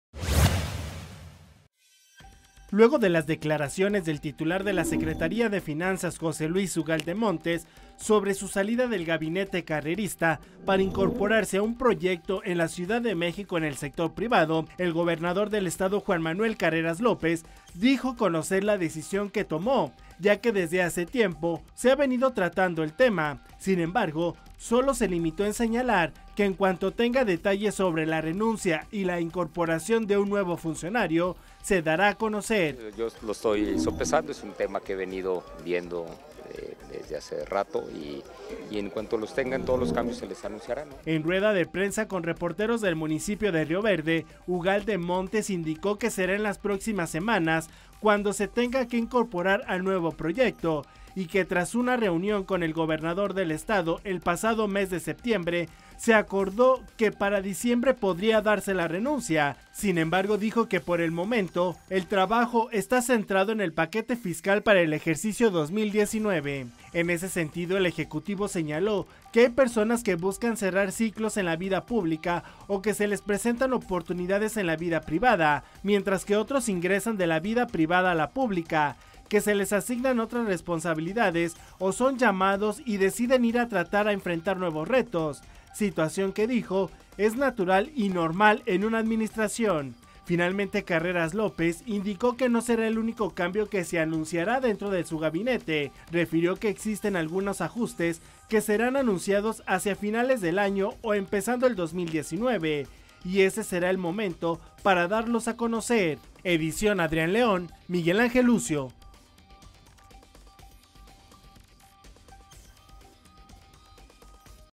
Reconoce JMCL la salida de Ugalde
Luego de las declaraciones del titular de la Secretaría de Finanzas, José Luis Ugalde Montes, sobre su salida del gabinete carrerista para incorporarse a un nuevo proyecto en la Ciudad de México en el sector privado, el Gobernador del Estado, Juan Manuel Carreras López dijo conocer la decisión que tomó el fumcionario ya que desde hace tiempo se ha venido tratando el tema, sin embargo, solo se limitó en señalar que en cuanto tenga detalles sobre la renuncia y la incorporación de un nuevo funcionario, se dará a conocer.